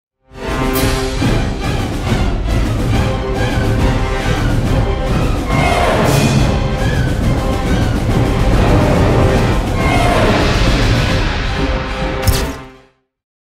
Victorian Robot Fight
A sword fight between a guy with a sword and a steam powered robot.